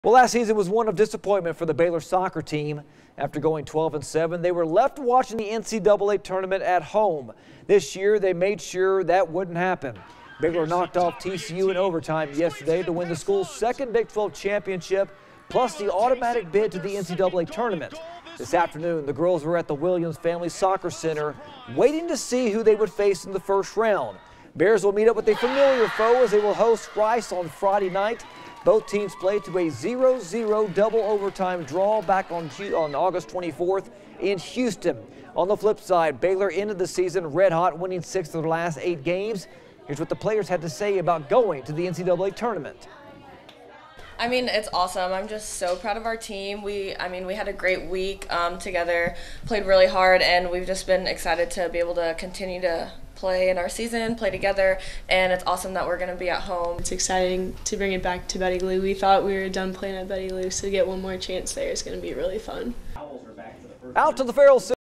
Baylor Soccer heads to NCAA Tournament
Last season was one of disappointment for the Baylor Soccer team.